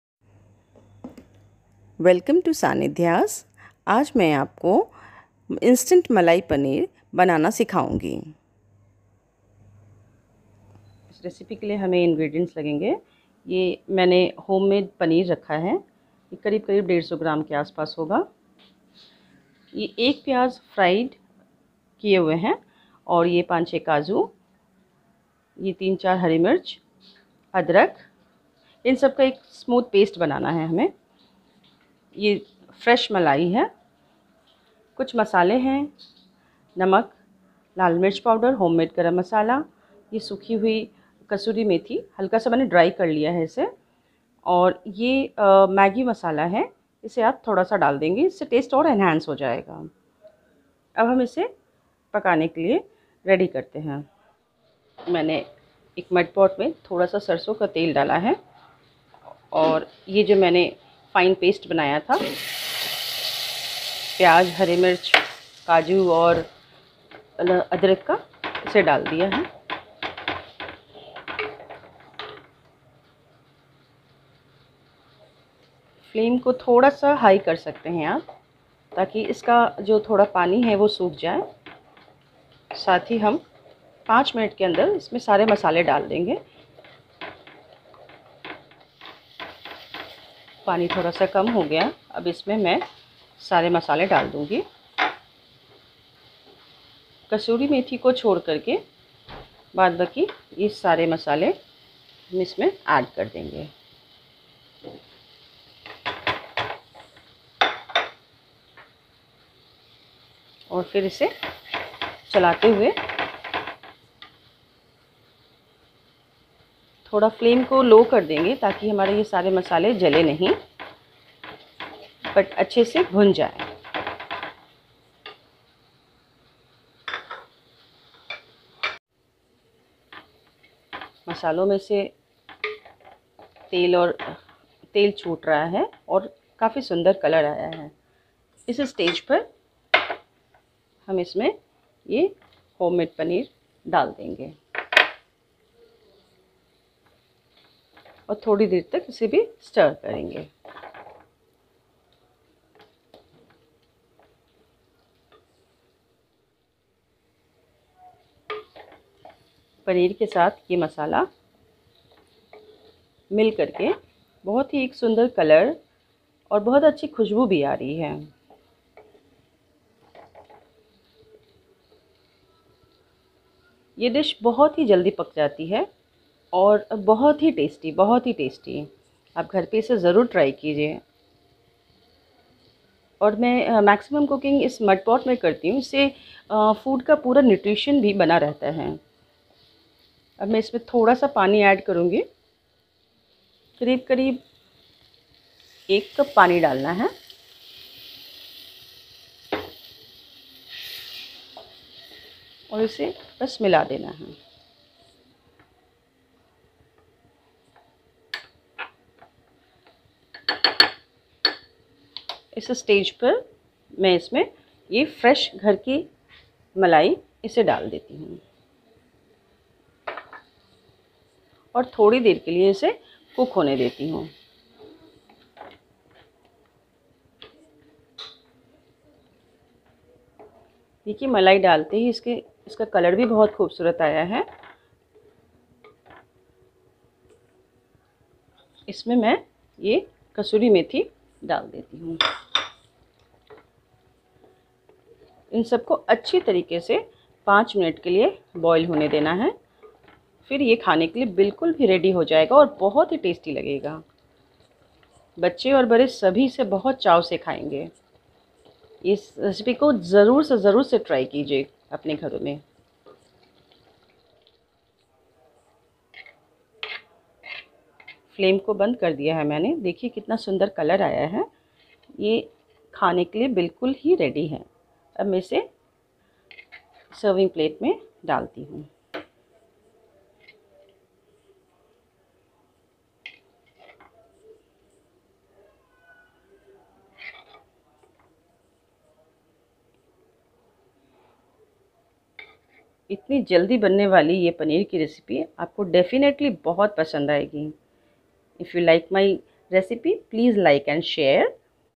Instant Malai Paneer
Ingredients:‐
• Homemade Paneer 150 gm.
• One onion sliced and deep fried. 🧅
• 4‐5 cashews, 3‐4 green chillies, 1/2 inch ginger.
• 1/2 cup fresh cream.
• 1 tsp red chilli powder🌶, 1tsp maggi magic masala, 1 tsp roasted kasuri methi, 1/2 tsp homemade garam masala and salt to taste.

Procedure :‐
• Make a fine paste of cashews, green chillies, fried onion & ginger.
• In a mud pot put 1 tsp mustard oil, pour the paste of cashews, green chillies,  fried onion & ginger.
• After frying the paste put all the masala into the pot except kasuri methi.
• When oil oozes out, put the paneer and toss a bit.
• Pour 1 cup water and mix the malai and kasuri methi and let the curry boil for 3‐4 minutes.
• Instant Malai Paneer is ready in 10 minutes.
Enjoyyyyy !!!